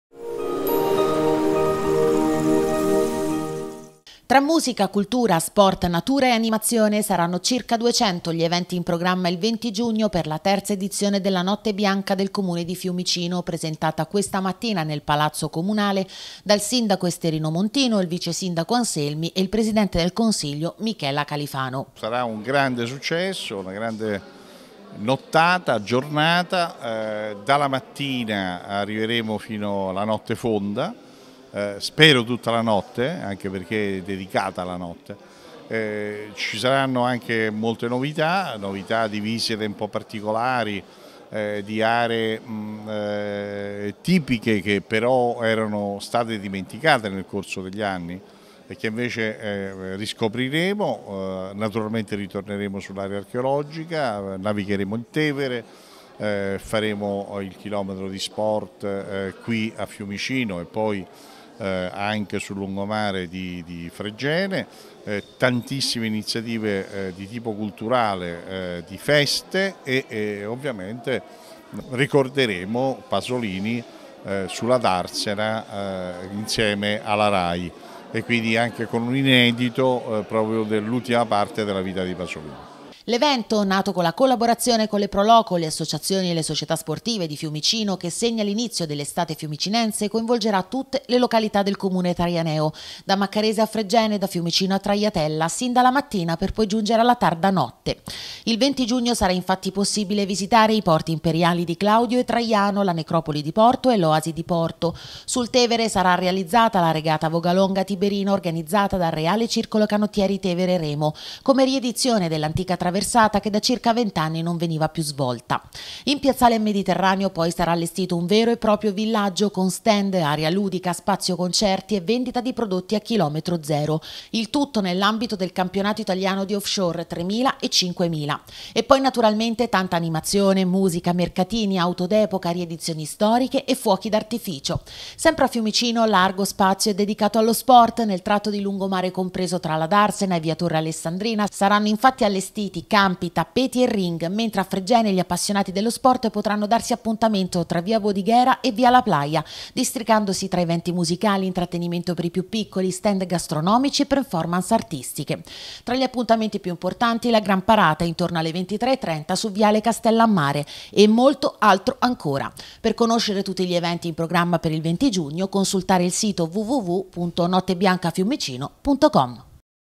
Fiumicino, oltre 200 eventi per la "Notte bianca" 2015
Si ricorda che il cartellone della Notte Bianca sarà in costante aggiornamento fino al 20 giugno per cui il programma dettagliato è consultabile sul sito Le attività interessate a partecipare con degli eventi o che propongono menù a prezzi particolari possono inviare una mail all’indirizzo di posta elettronica